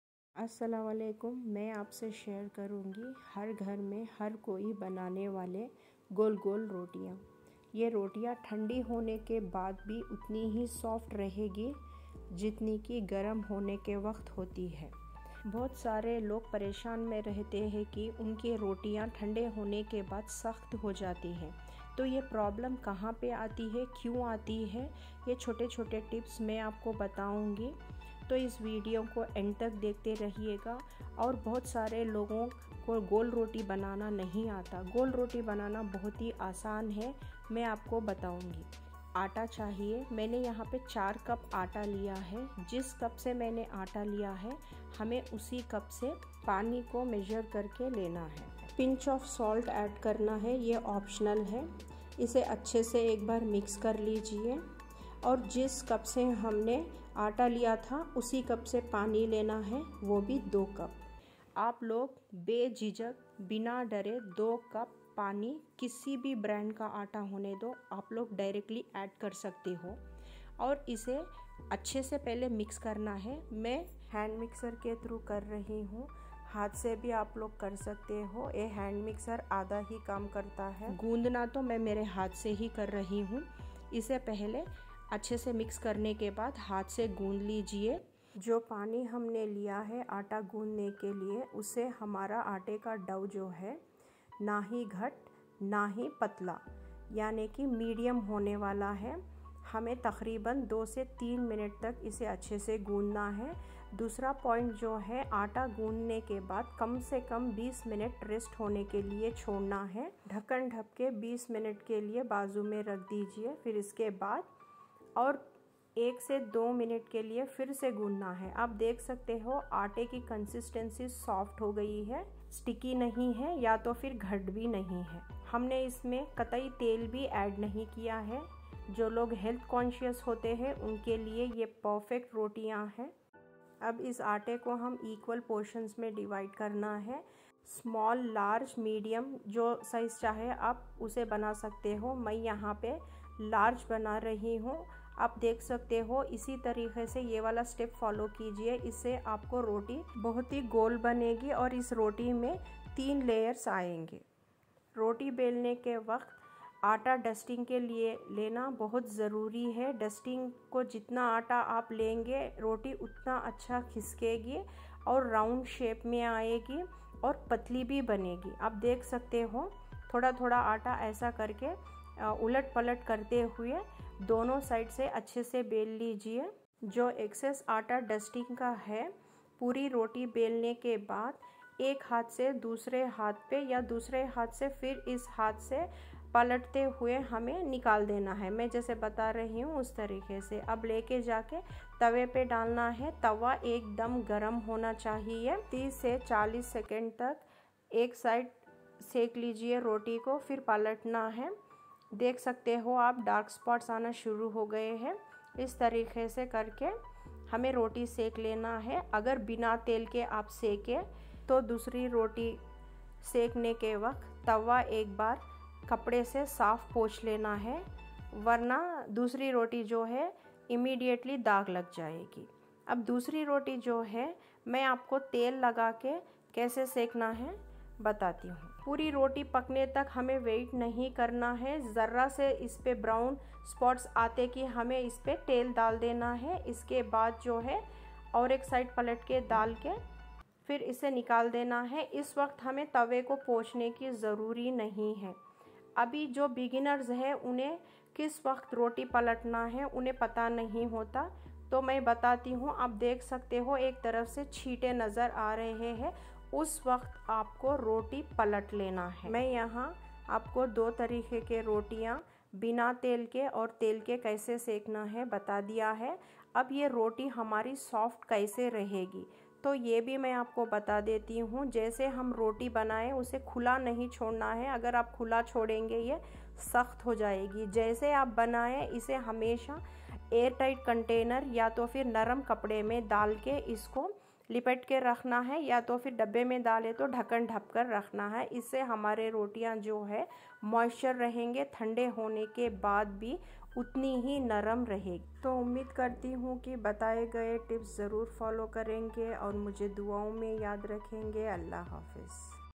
roti / phulka | super soft roti for rich gravy curries by aieshaskitchen
how to make super soft roti or phulka for rich gravy curries by aieshaskitchen